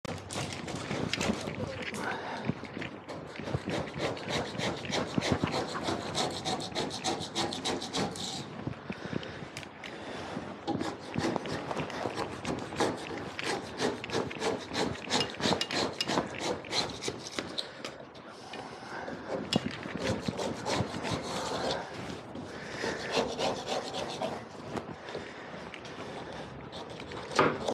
6 May 2023
Scraping dried on guano from beams